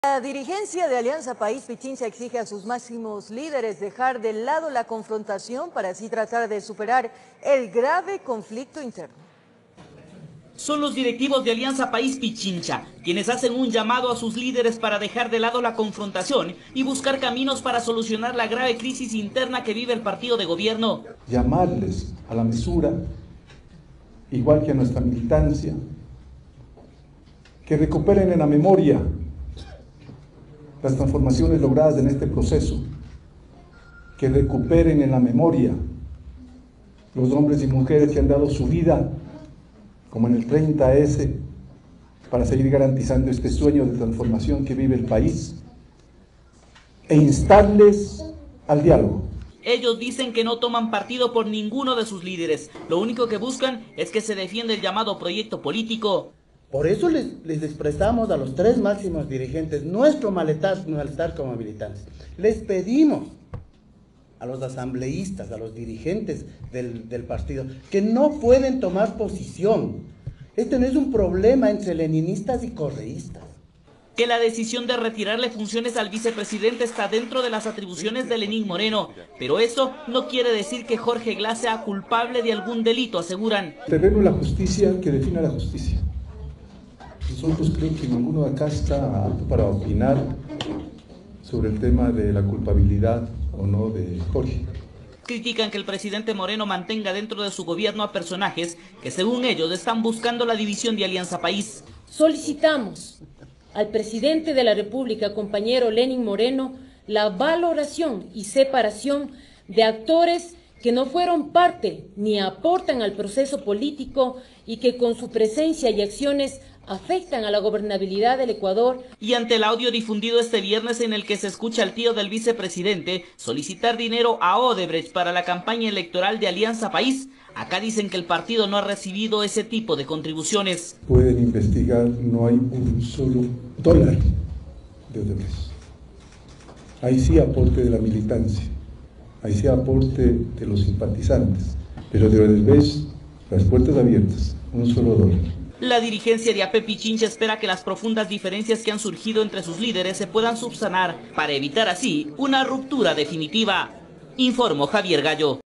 Dirigentes de Alianza País Pichincha pide dejar de lado las confrontaciones
El dirigente, Gustavo Baroja, entregó una rueda de prensa en donde pide a su partido político que evite las confrontaciones y lleguen a puntos comunes.